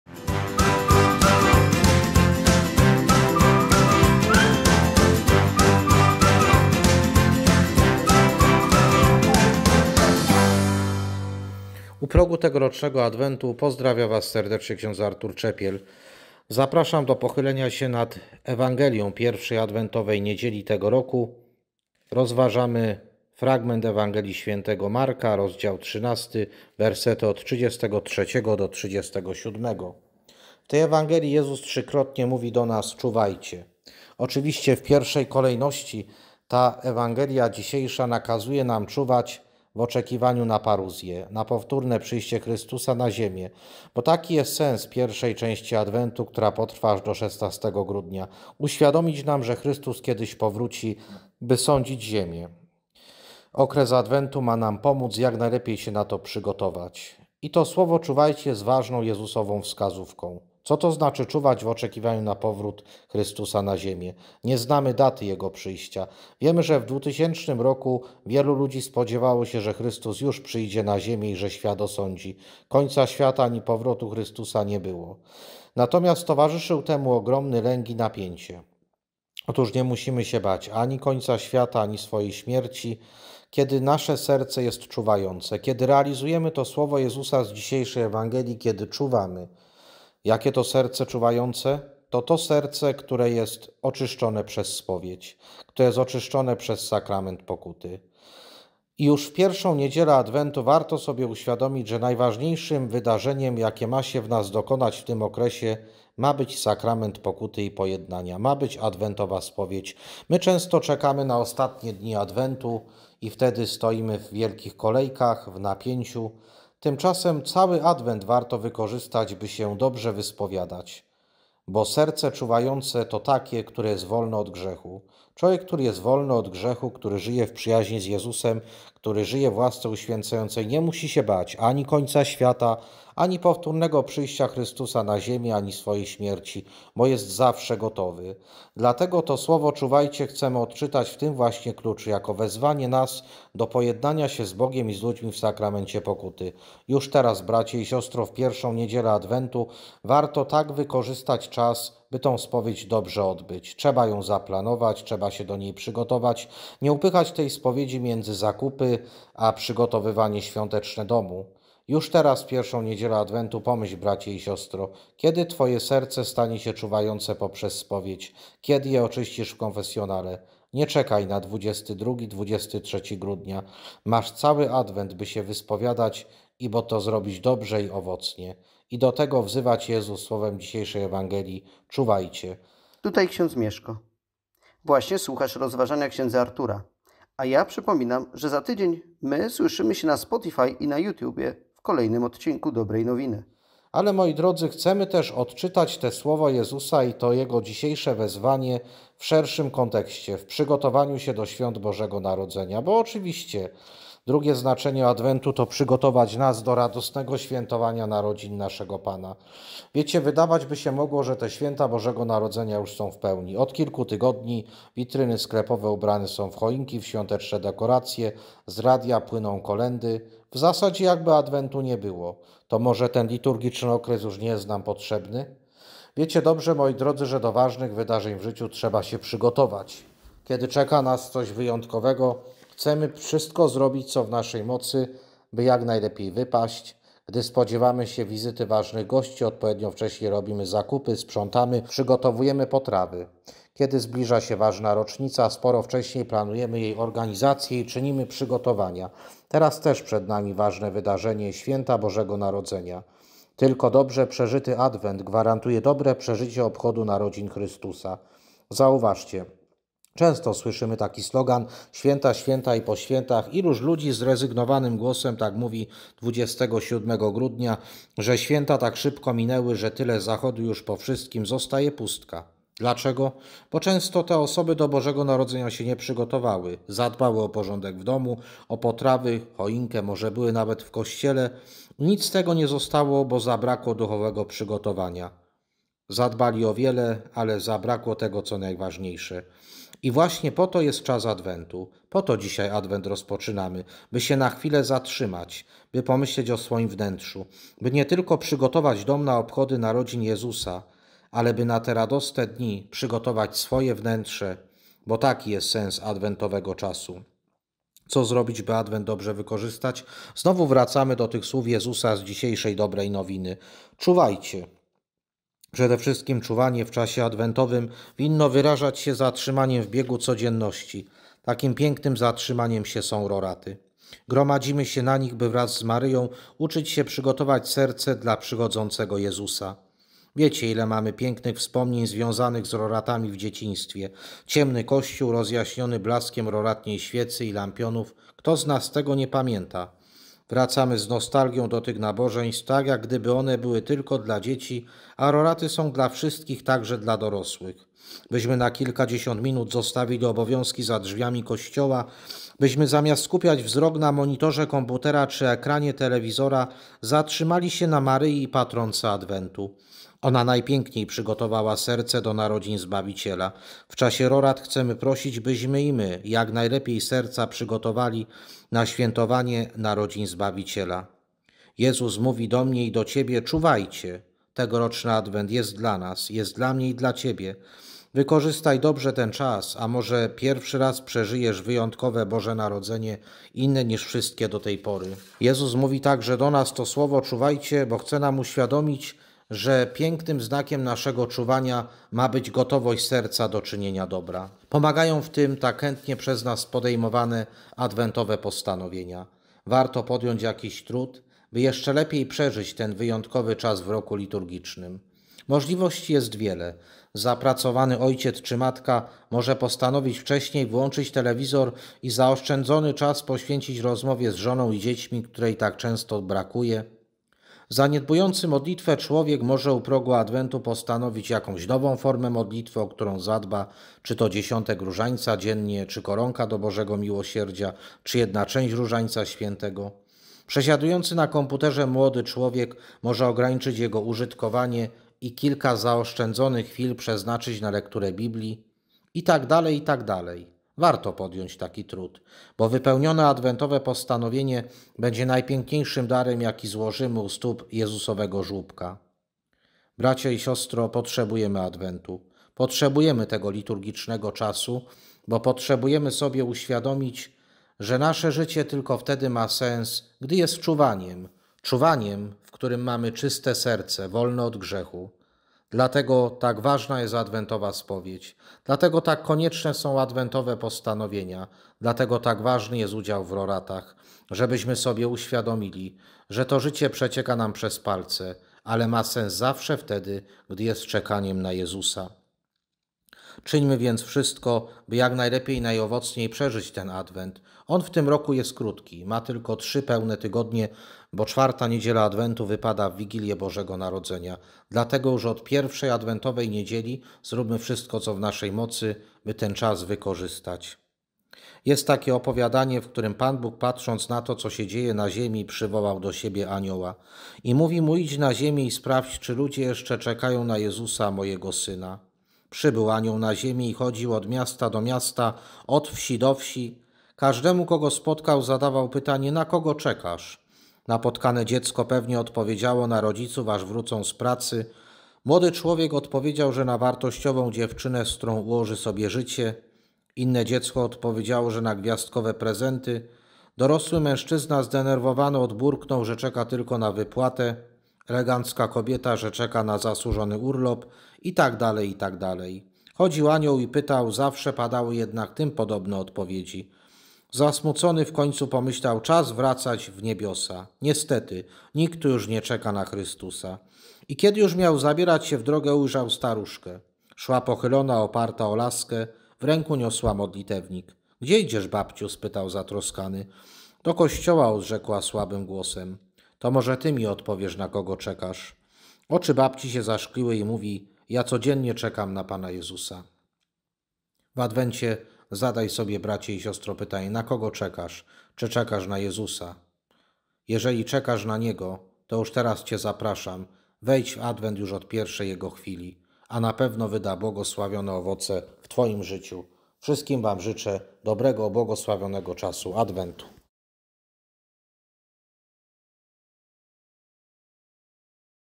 Dobra Nowina EXTRA s04 e01 – Czuwajcie!
Rozważanie na I Niedzielę Adwentu roku B.

Fragment Ewangelii do rozważenia (Mk 13, 33 - 37):
"Uważajcie, czuwajcie, bo nie wiecie, kiedy czas ten nadejdzie. 34 Bo rzecz ma się podobnie jak z człowiekiem, który udał się w podróż. Zostawił swój dom, powierzył swym sługom staranie o wszystko, każdemu wyznaczył zajęcie, a odźwiernemu przykazał, żeby czuwał. 35 Czuwajcie więc, bo nie wiecie, kiedy pan domu przyjdzie: z wieczora czy o północy, czy o pianiu kogutów, czy rankiem. 36 By niespodzianie przyszedłszy, nie zastał was śpiących. 37 Lecz co wam mówię, do wszystkich mówię: Czuwajcie!»"

realizacja: ks. Artur Czepiel
montaż: Piotr Ćwik